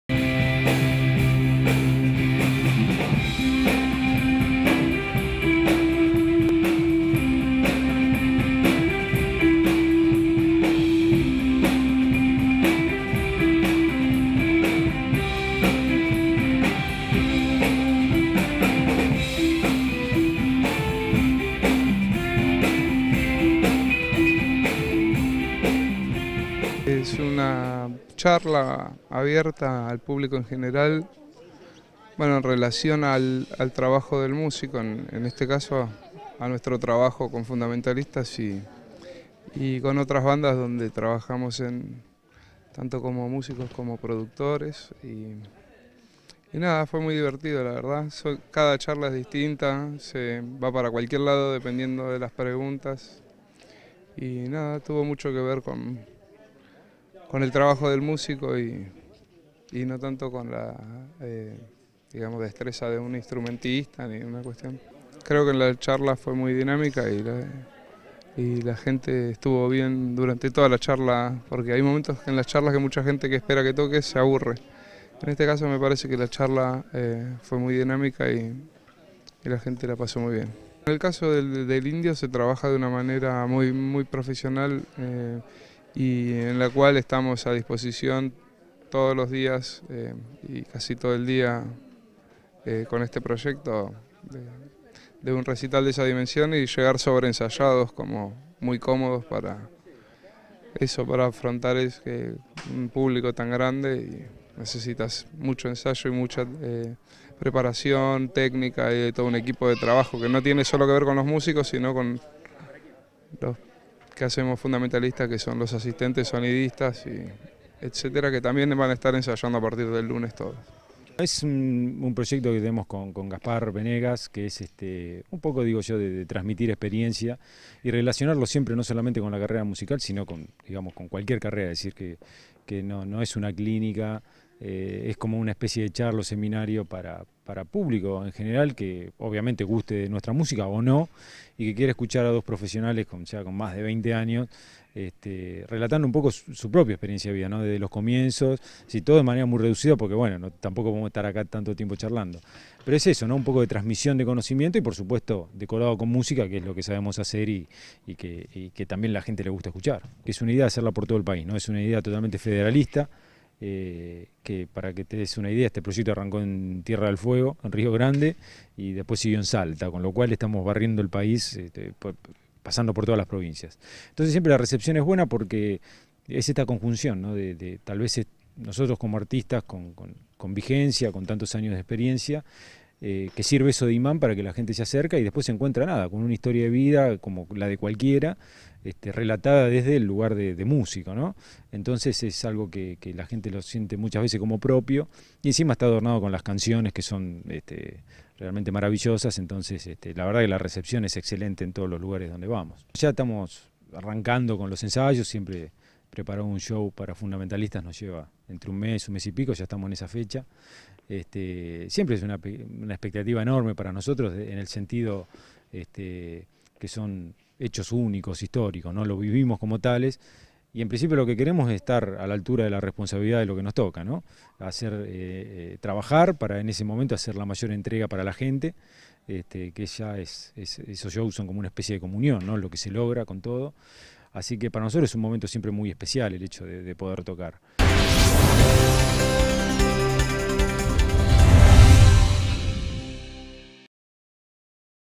Música y charlas para el público del Futurock
Gaspar Benegas y Hernán Aramberri, integrantes de Los fundamentalistas del Aire Acondicionado, la banda del Indio Solari, dictaron una clínica sobre música para el público asitente del Futurock.

Frente a una audiencia muy atenta, los músicos compartieron sus más de 20 años de experiencia como profesionales del rubro.

Luego en diálogo con Télam, adelantaron lo que será el próximo recital del Indio Solari, el 13 de diciembre en el Autódromo Jorge Ángel Pena en la localidad de San Martín, provincia de Mendoza.

La fecha fue confirmada a través de RedonditosdeAbajo, el canal oficial elegido por Solari para comunicar todas las novedades de su carrera.